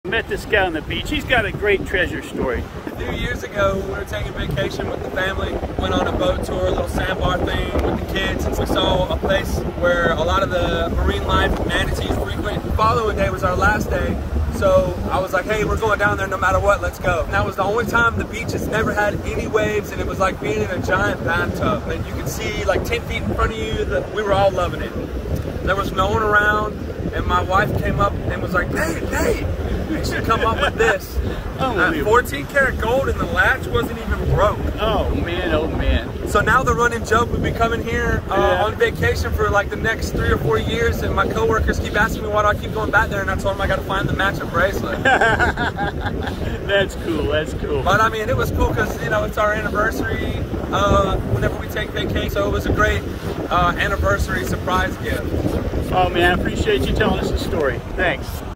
Beach Treasure Tales Incredible Anniversary Gold Chain found
2023. 1st of a series Beach Treasure Tales, told to me by folks I meet out on the beach. This guy and his family met me while I was metal detecting.